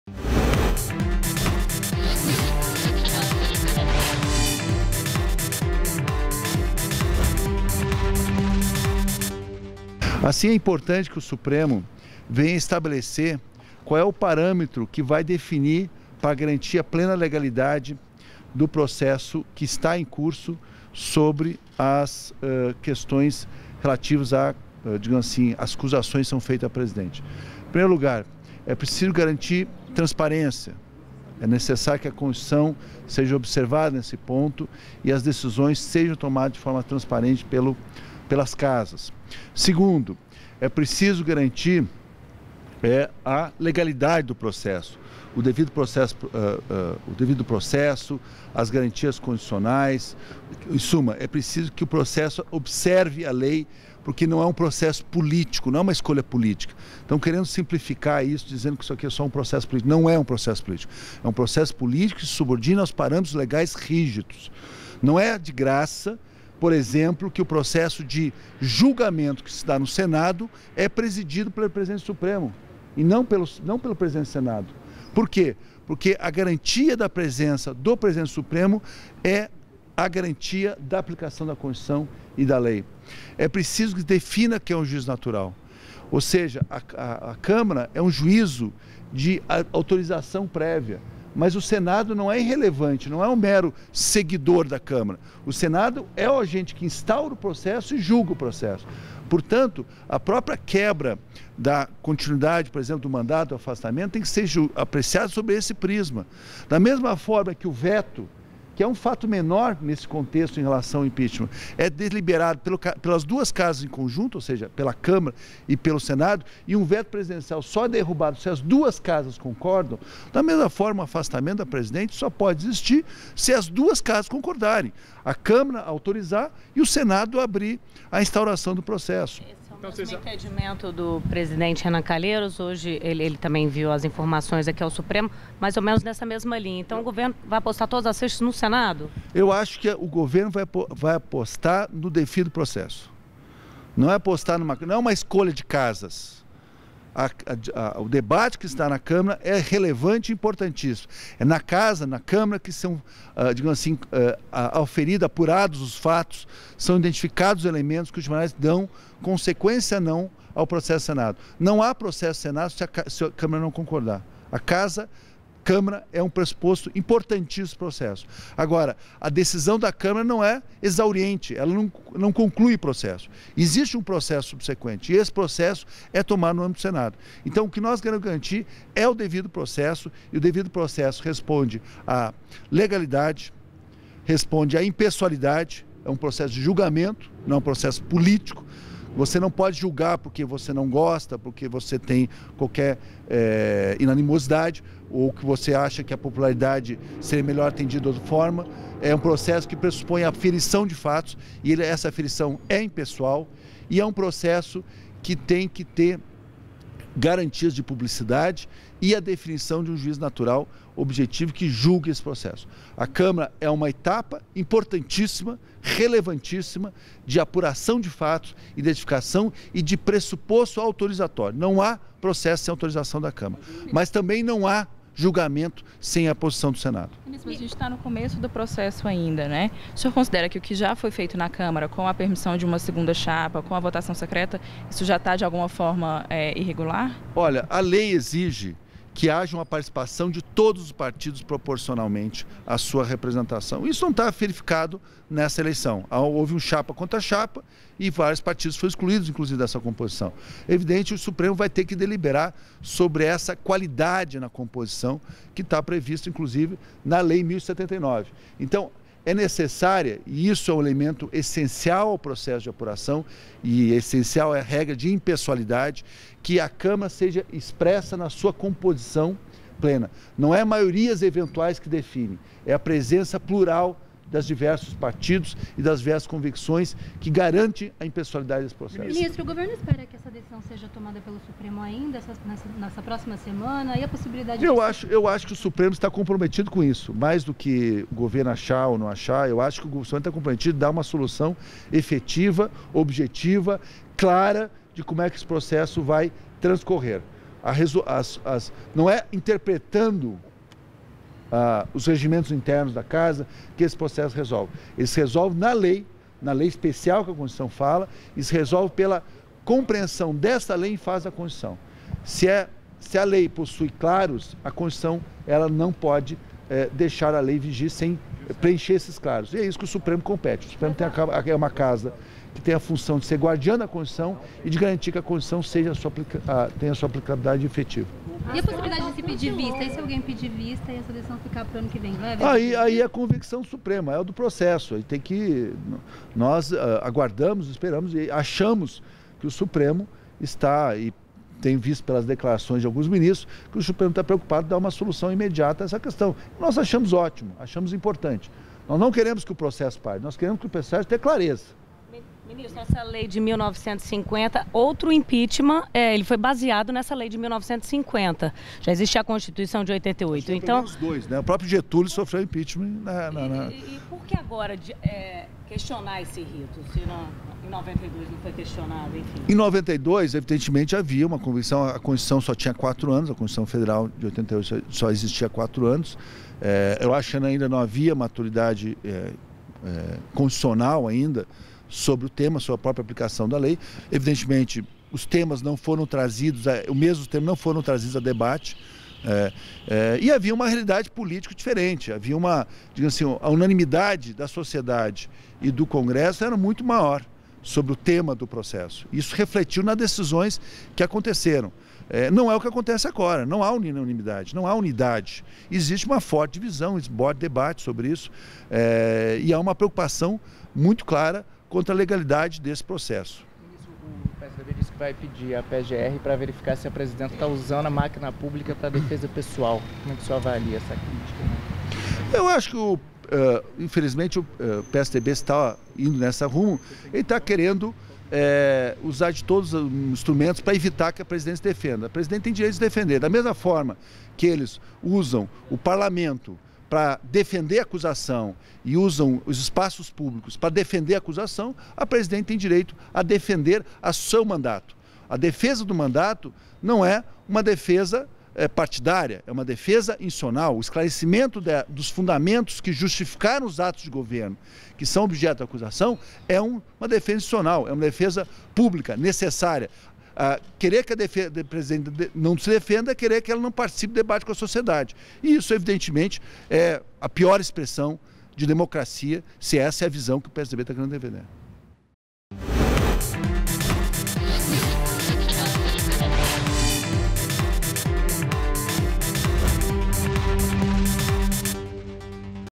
Governo defende no STF que impedimento seja decidido no Senado
NOTÍCIAS DA FONTE - 11.12.15: Advogado Geral da União,  Luís Inácio Adams, fala em entrevista sobre a importância de o STF​ estabelecer qual parâmetro vai definir. "É preciso garantir transparência e legalidade na análise do impedimento.